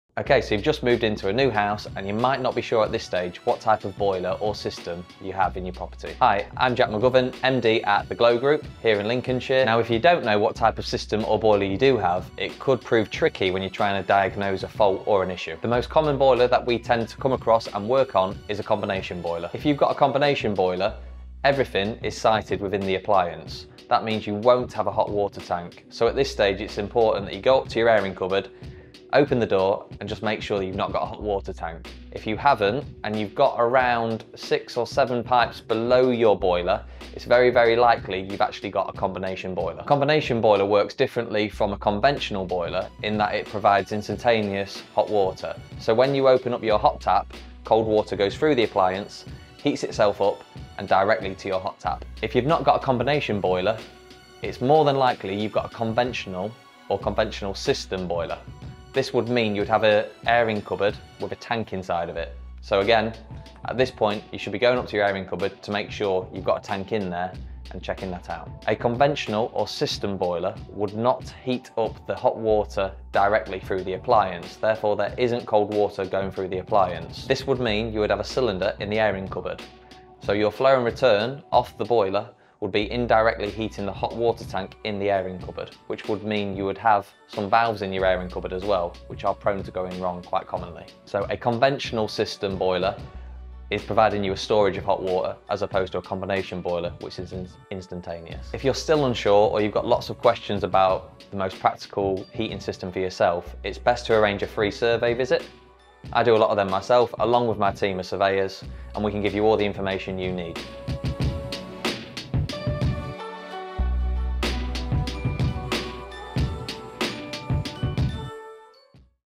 What boiler do I have? Things to look out for.
Not sure on what type of boiler you have? Jack explains the most common types of boiler, the differences between them, and how you can spot which you have.